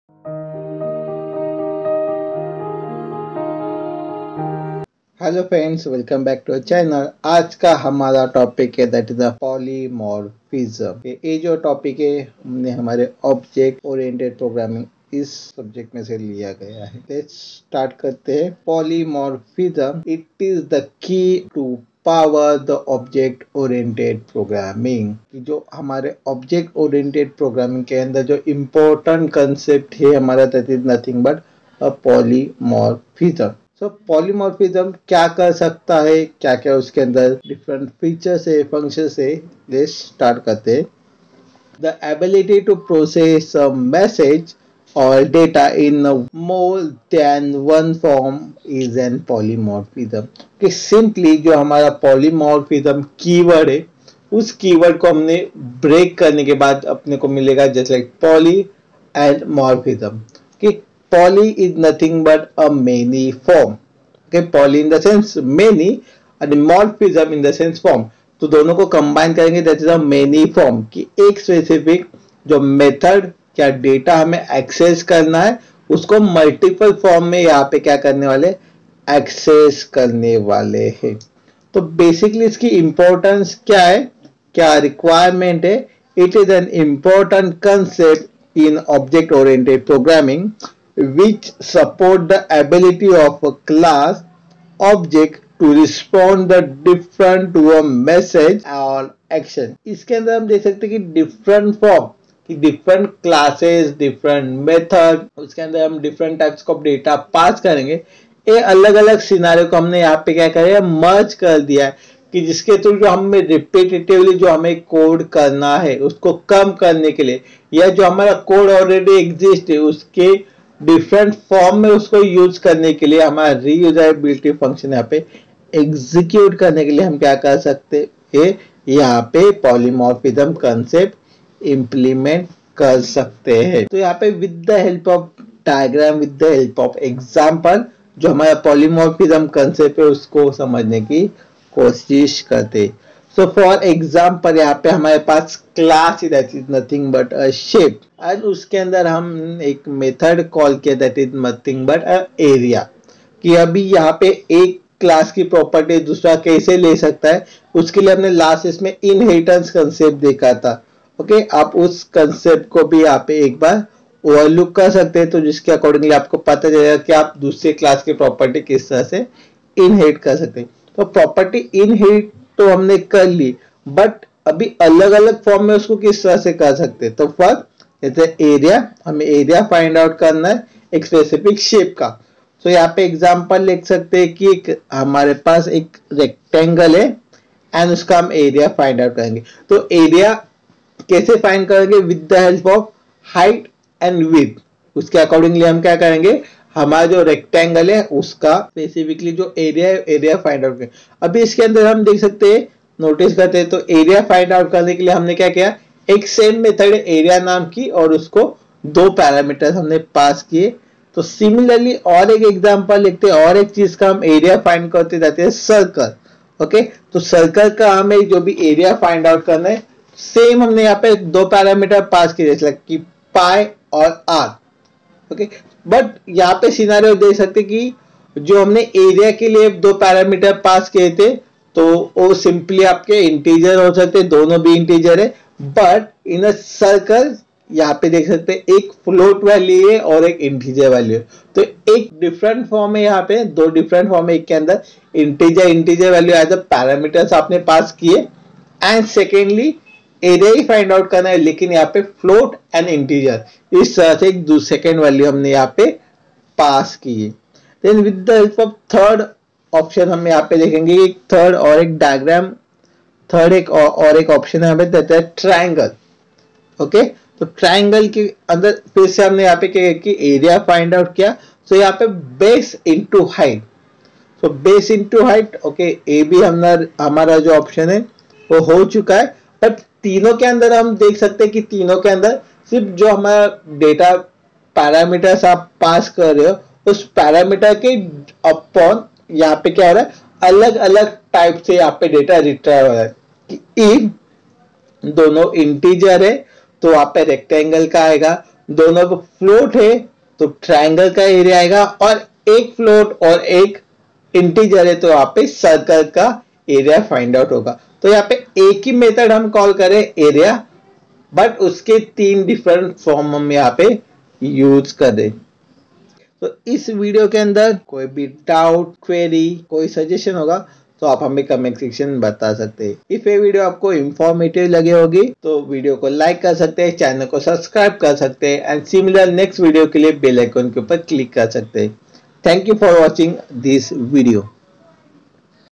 LECTURE 08 :- POLYMORPHISM IN OBJECT ORIENTED PROGRAMMING
The ability to process a message or data in more than one form is polymorphism.
It  is an important  concept in object oriented programming which support the ability of a class object to respond differently to a message or action.
Consider ourselves as a real-life example.

Link for slides, code samples and text version of the video

Chapter
0:00 Start
0:20 Polymorphism
2:15 Example